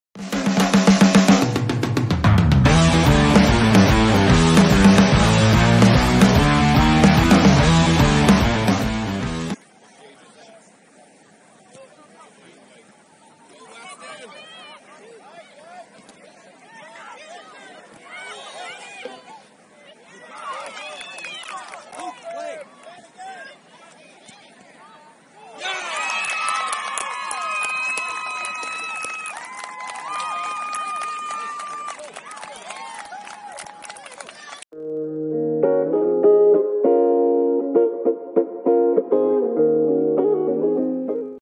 Barrie SC U11 x Greece United FC Goal #38
U11 Boys Barrie SC x Greece United FC on July 8, 2023
1st Game at the Cobras FC Summer Classic Tournament in Rochester, NY

A&W High Voltage Contracting LTD
LHG Photography
Mapleridge Power & Lighting Company Limited